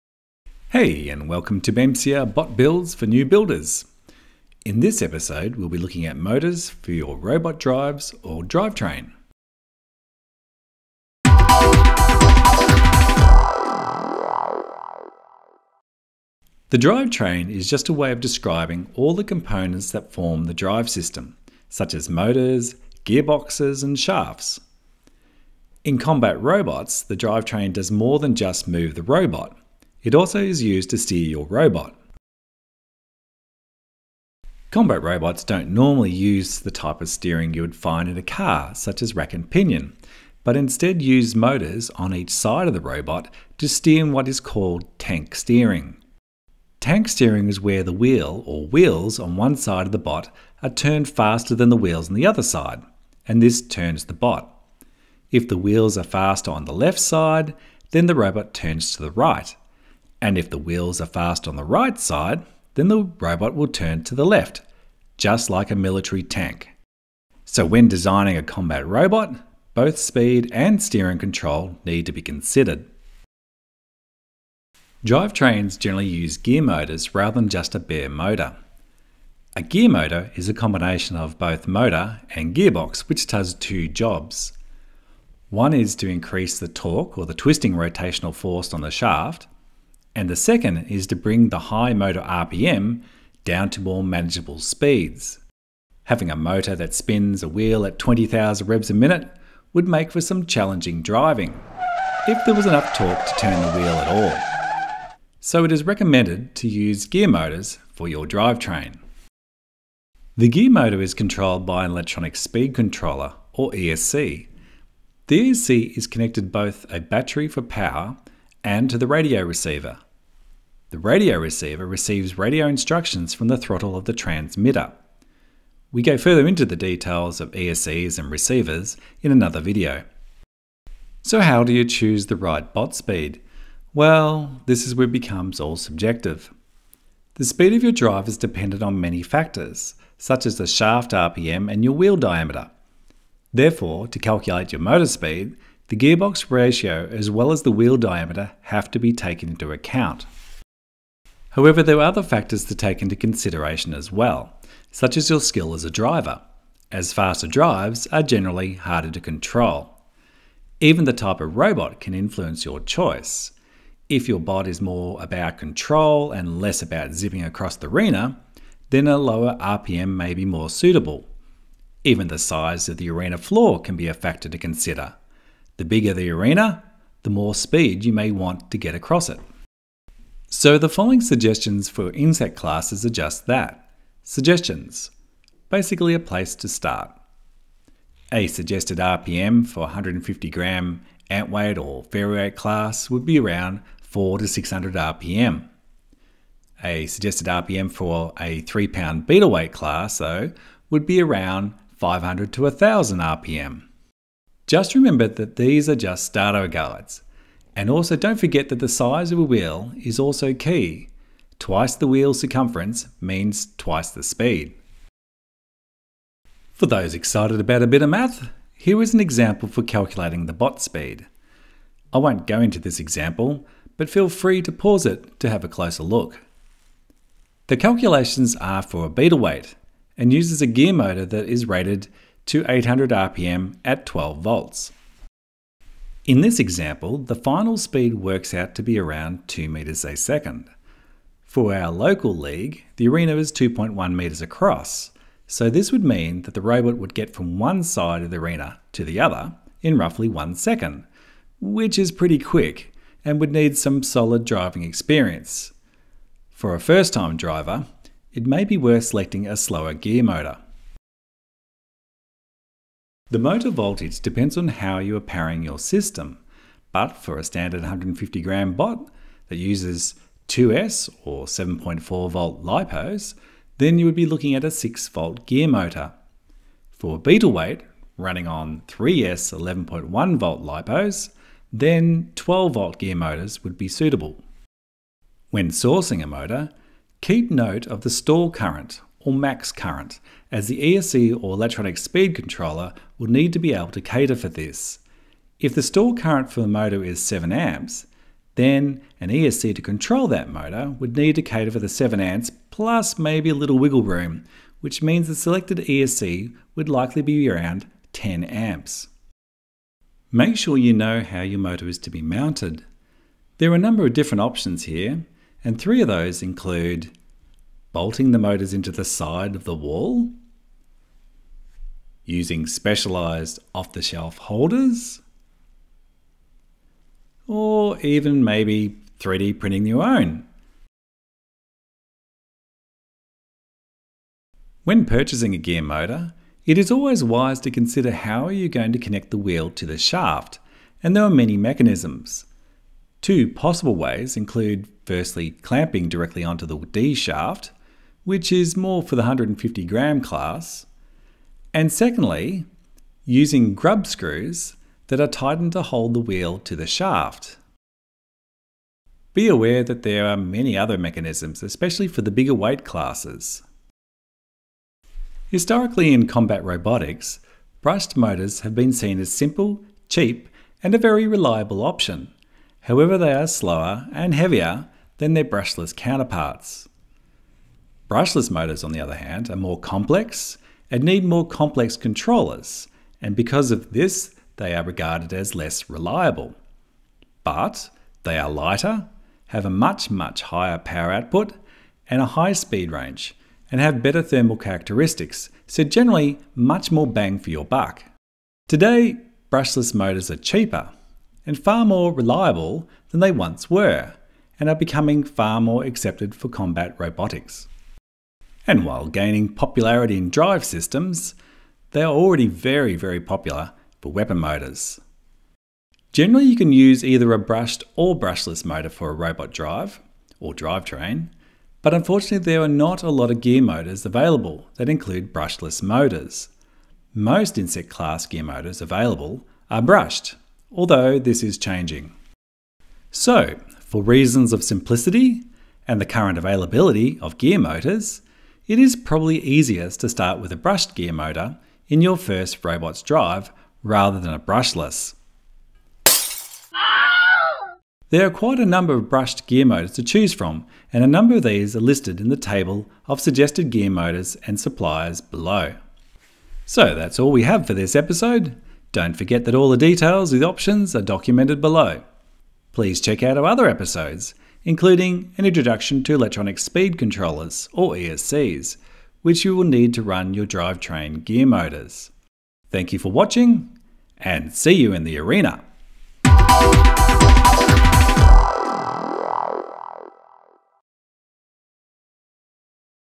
How to: Robot Drive Gearmotors for your Combat Robot
A starting place for new combat robot builders for Brushed and Brushless drive motors, and Electronic Speed Controllers (or ESCs), Mounts, and speeds.  Tips and Tricks for Combat Beetleweight, Antweight and Featherweight Classes.  Presented by Melbourne Combat Robotics.